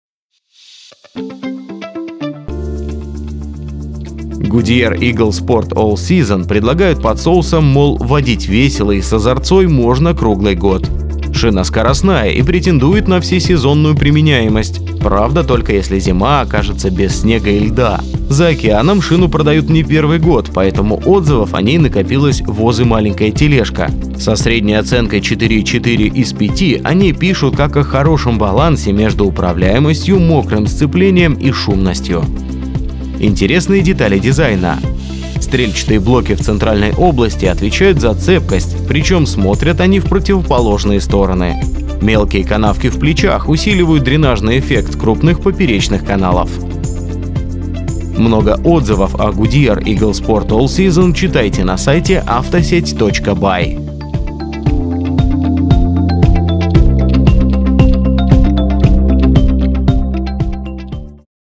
Goodyear Eagle Sport All Season ❄️☀️: обзор шины и отзывы ● Автосеть ●
Отзывы о Goodyear Eagle Sport All Season, размеры и стоимость шины

Переходите по ссылке, чтобы узнать, в какой стране произвели Goodyear Eagle Sport All Season, какие оценки у этой шины в евроэтикетке, какая у неё начальная глубина канавок и сколько она весит.